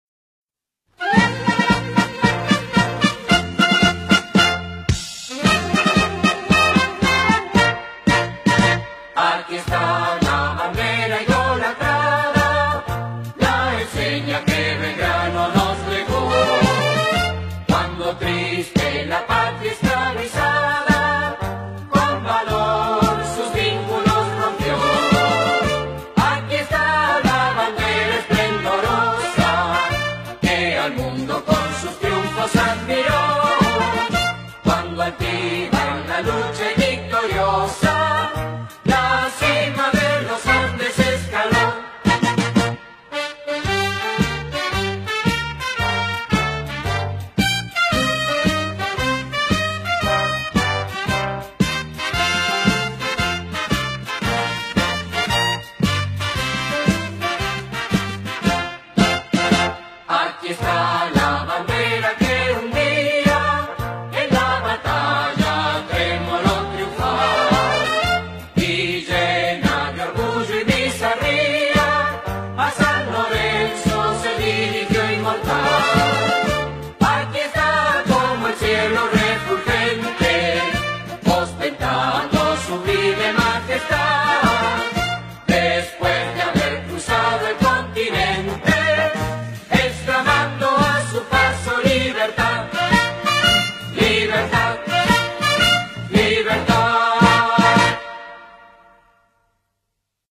saludo a la bandera
mi primita dolores con 4 añitos juro la bandera por primera vez y con la mano en el corazon que orgullo mas grande saber que mis primos siguen mis pasos